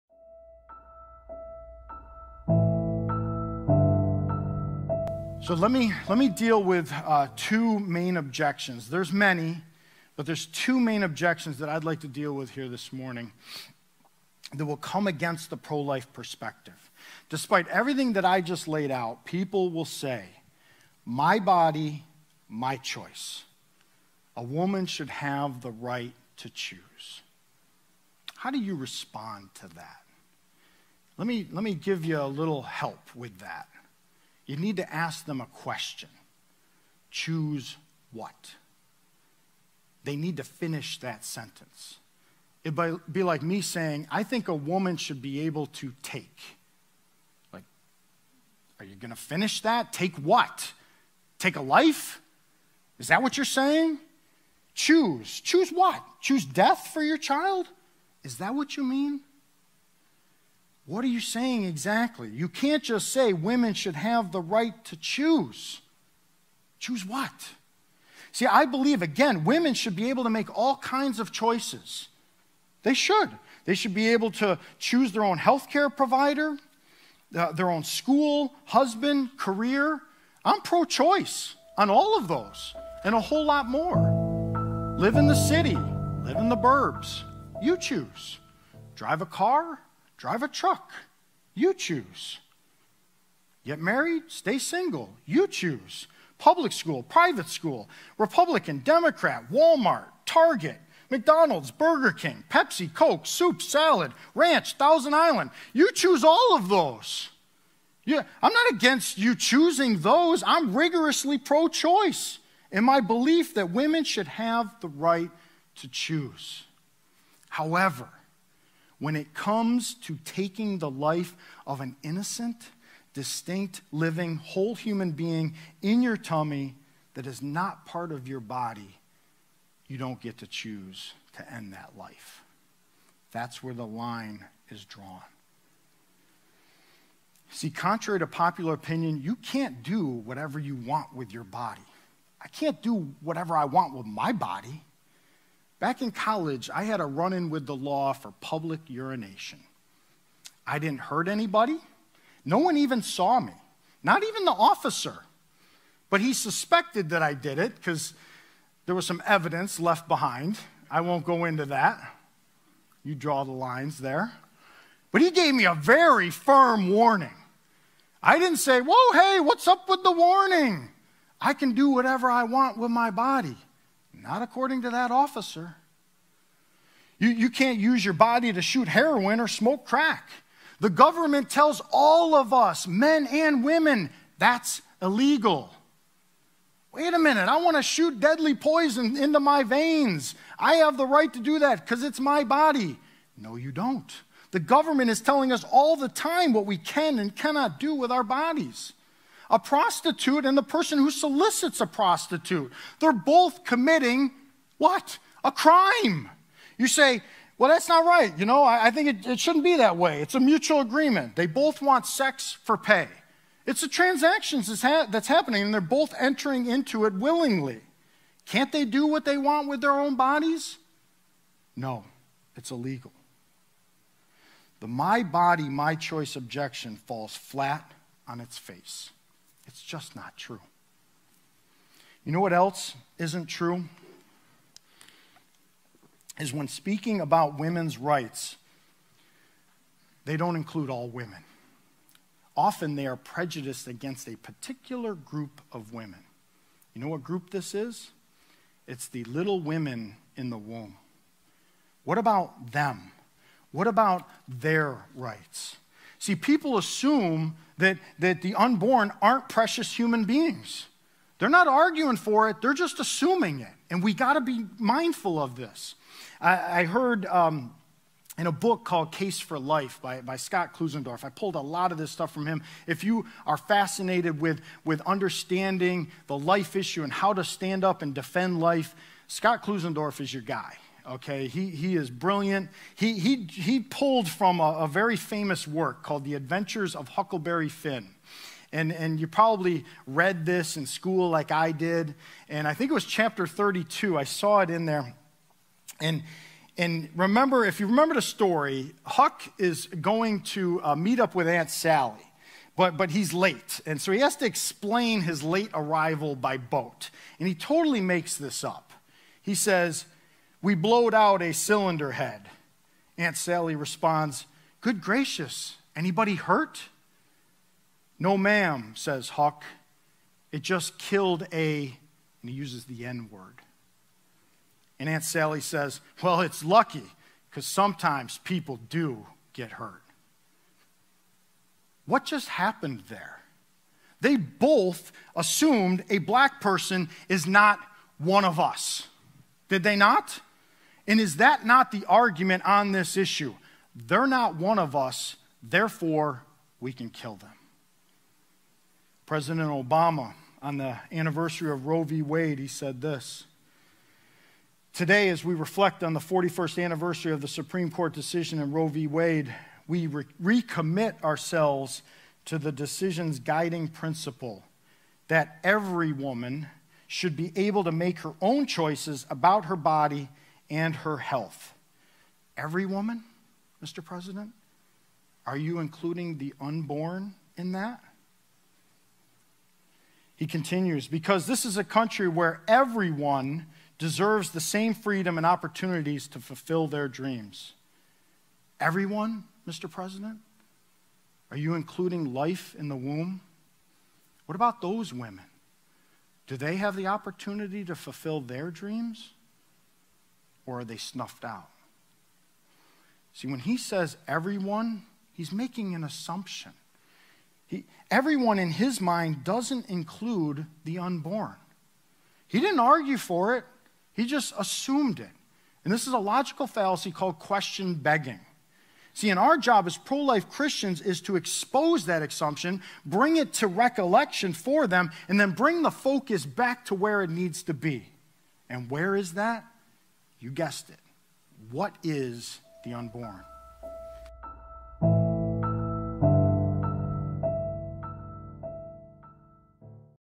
Defend Life - Part 2 - OBJECTION: My Body, My Choice
Dealing with the first of two major objections that face the pro-life position.

CHH Lyric Videos, Theology, Apologetics, Evangelism!

ENGAGE Gospel Outreach provides CHH Lyric Videos, as well as sermons and other videos on apologetics and evangelism.

(CHH stands for Christian Hip Hop)